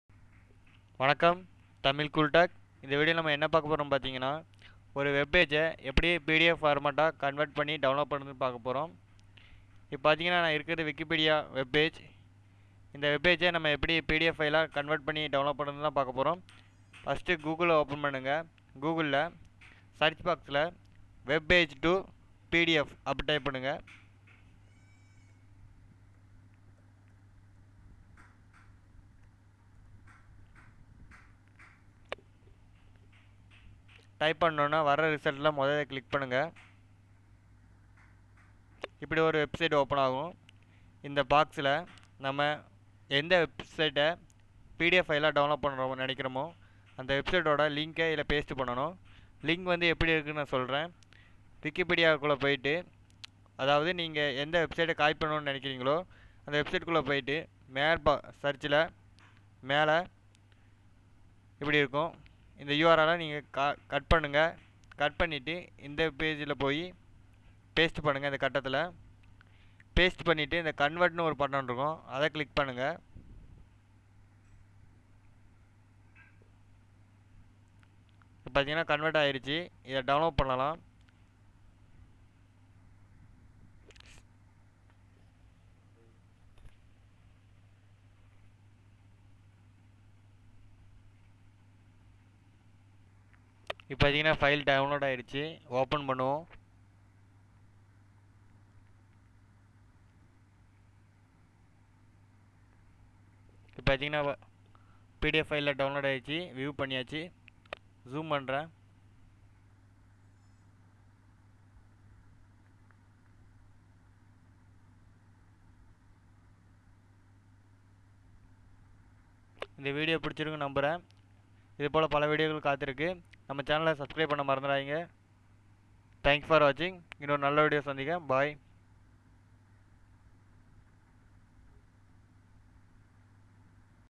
How to Download Any Web page As PDF Tamil
in this video you will learn in web pages  as pdf format file in download without any softwares ... How to Download Any Web page As PDF Tamil